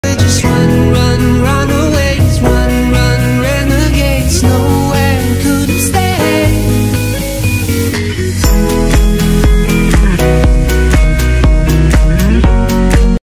Van build: shortening our swivel seats (timelapse)
Our swivel seats were a bit too high to be able to sit comfortably, so we bought lower frames to install